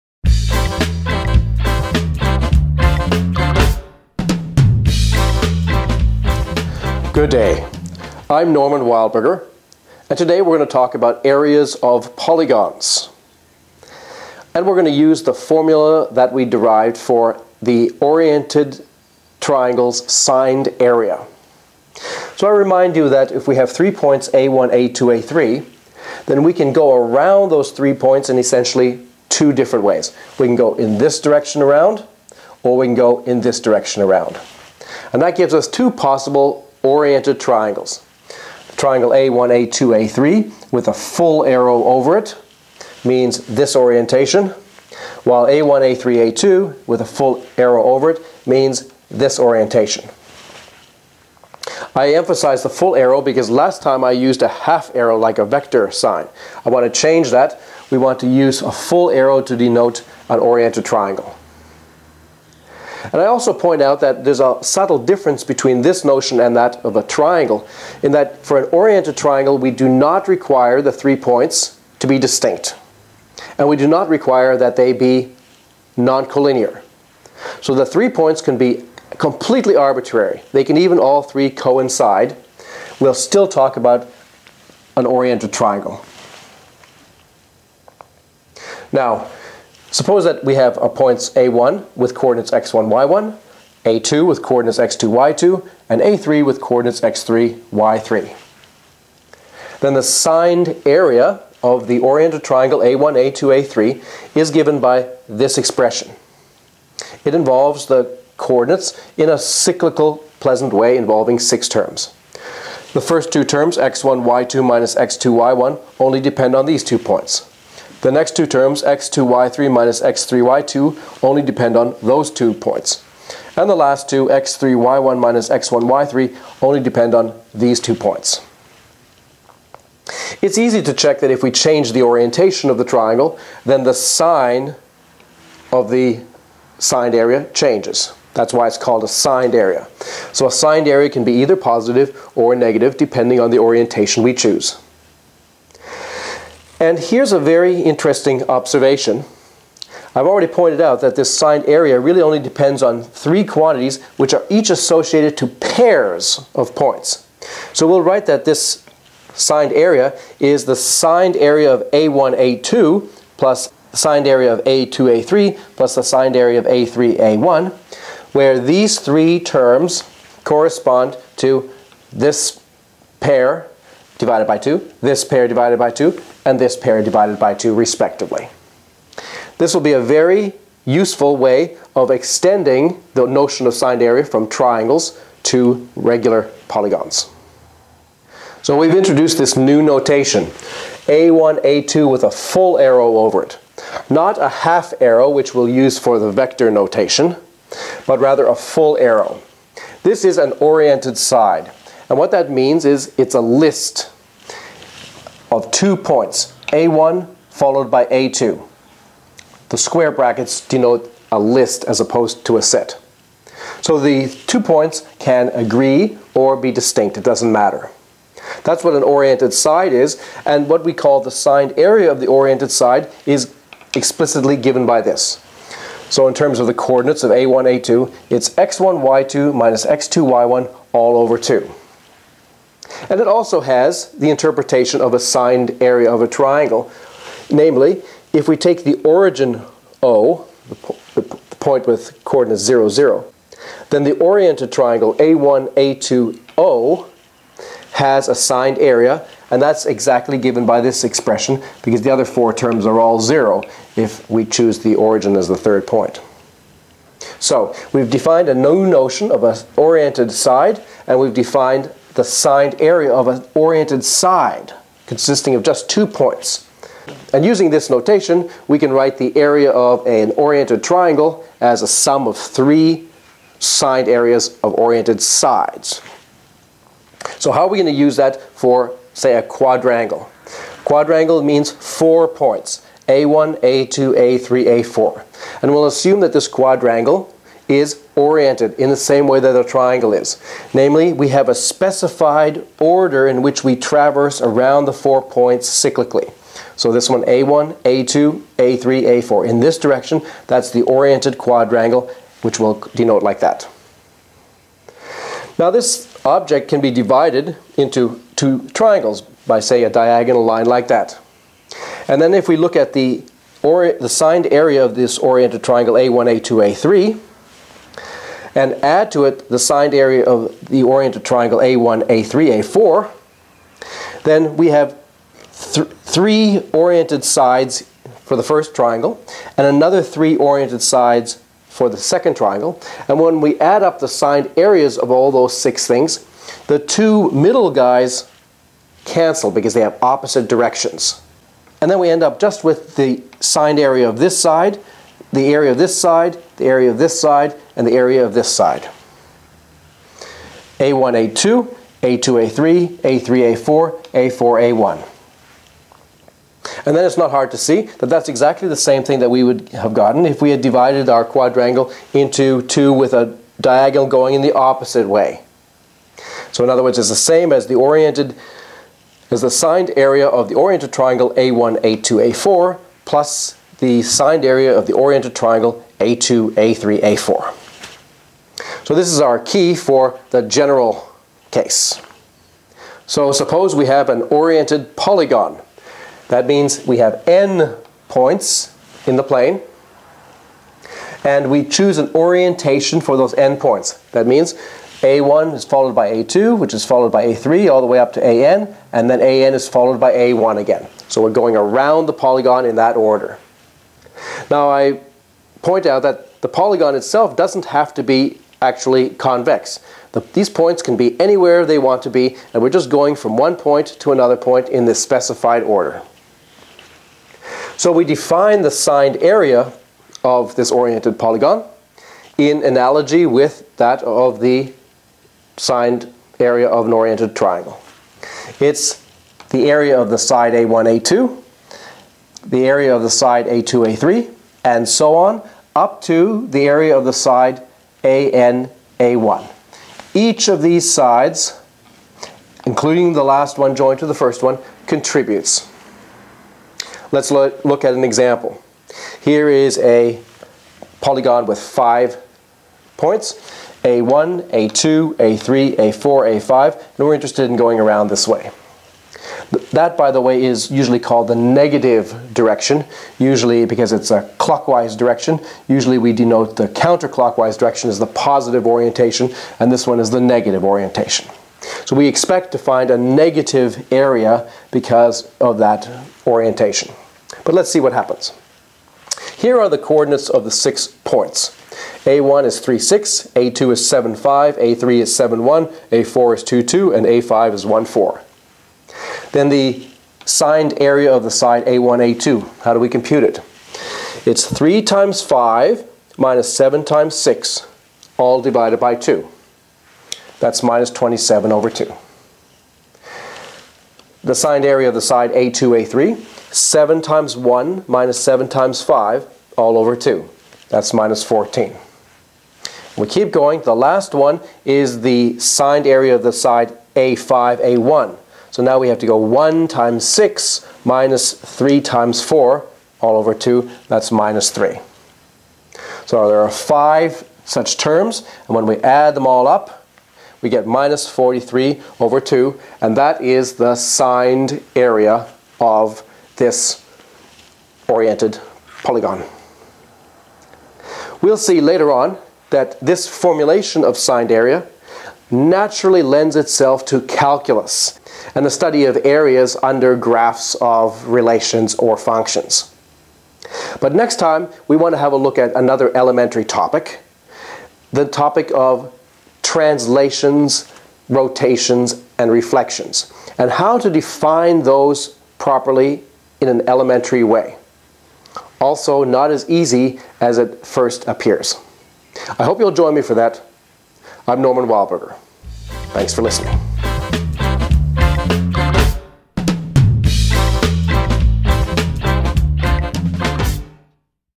Areas of polygons | Arithmetic and Geometry Math Foundations 34 | N J Wildberger
How to define the area of a polygon? The right way is to consider signed areas of oriented polygons. This leads to natural formulas that are important for calculus.

This video belongs to Wildberger's MathFoundations series, which sets out a coherent and logical framework for modern mathematics.

Video Content:
00:00 Signed area of an oriented triangle (review)
3:08 Oriented sides and their signed areas
6:22 Oriented polygons and their signed areas